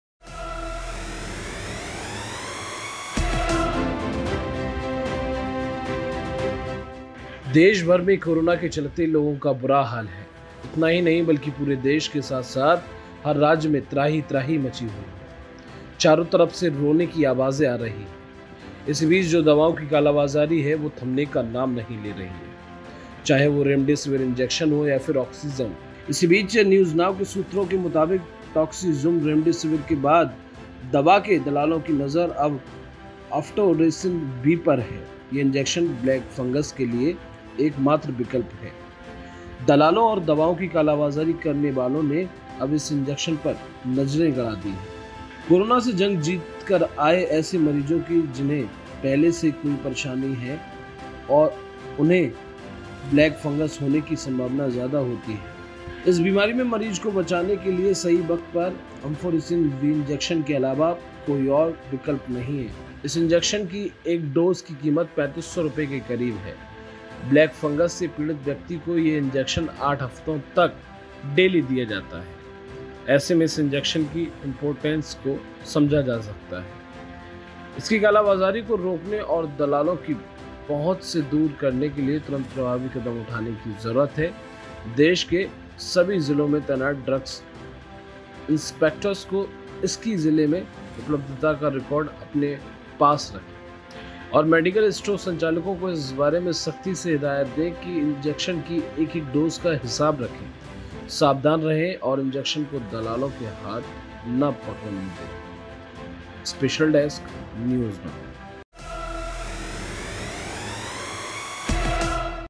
रेमडेसिविर के बाद दलालों की नज़र इस दवाई पर, देखिए।। NEWS NOW INDIA
देशभर में कोरोना के चलते लोगों का बुरा हाल है, इतना ही नहीं वल्कि पूरे देश के साथ साथ हर राज्य में त्राहि त्राहि मची हुई है, चारो तरफ से रोने की आवाज़ें आ रहीं हैं, इसी बीच जो दवाओं की  कालाबाज़ारी है वो थमने का नाम नही ले रही है, चाहे वो remdesivir इंजेक्शन हो या फिर ऑक्सीज़न,
इसी बीच न्यूज़ नाओ के सूत्रों के मुताबिक टॉक्सीज़ुम्ब, remdesivir के बाद दवा के दलालों की नज़र अब अम्फोटेरिसिन बी पर है, और यह इंजेक्शन मुकर्मीकोसिस ब्लैक फंगस के इलाज में प्रभावी है, सभी ड्रग इंसप से संबंधित कलेक्टर स्टॉक की जानकारी ले , इतना ही नही वल्कि तत्काल प्रशासन इस इंजेक्शन को नियंत्रड़ में ले, आप अंदाजा लगा सकते हैं, इस समय कितना खतरा है, आम आदमी को तो पता ही नही है कि चल क्या रहा है